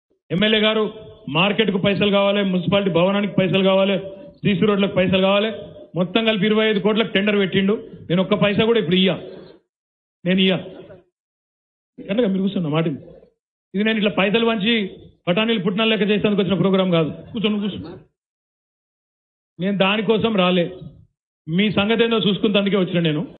Shocking behavior of Minister KTR.| TRS
Minister KTR shows his arrogance with his party MLA.